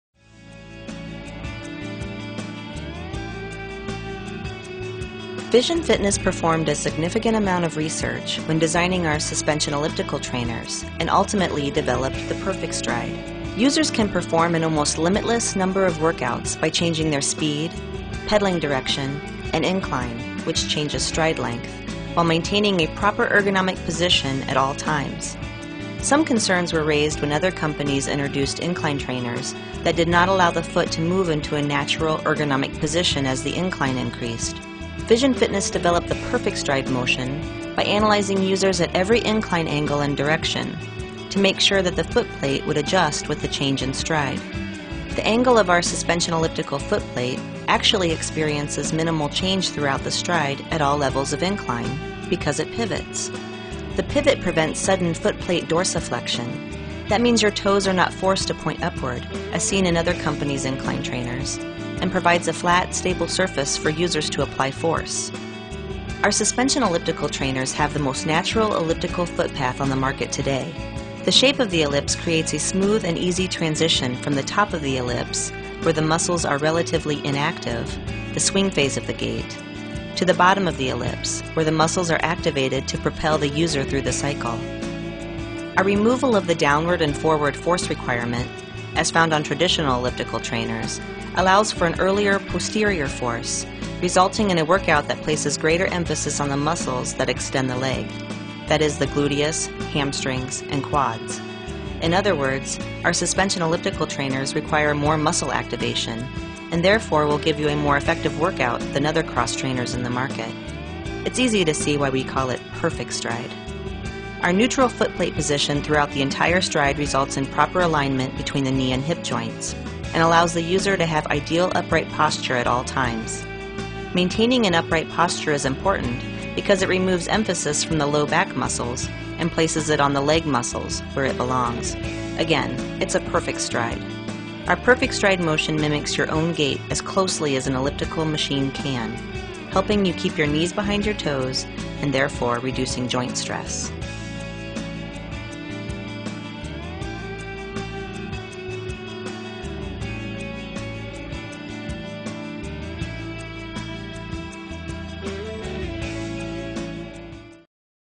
Vision FItness S7100 Suspension Elliptical Cross Trainer
Demonstration of the S7100 suspension ellipticals